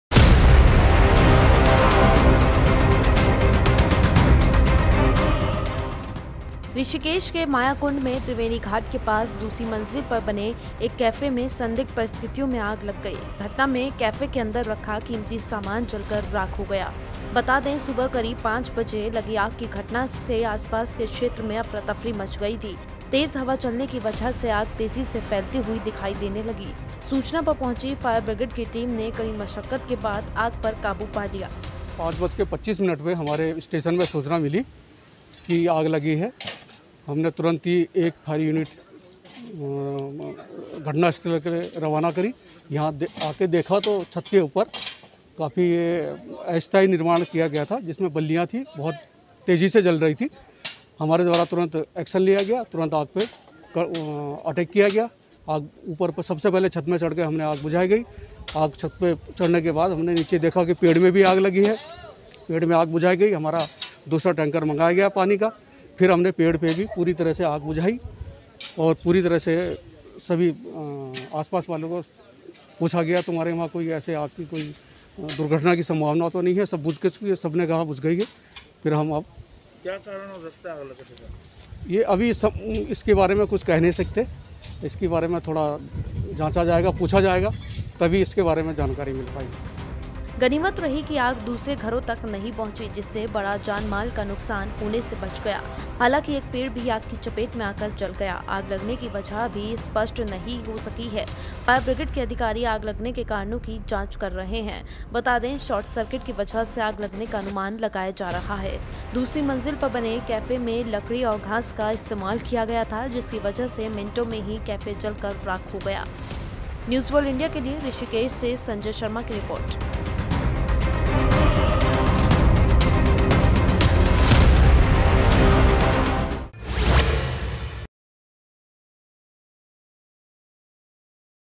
Rishikesh: Cafe में लगी भीषण आग fire की दो गाड़ियों ने पाया काबू
About This Video ►Rishikesh: Cafe में लगी भीषण आग fire की दो गाड़ियों ने पाया काबू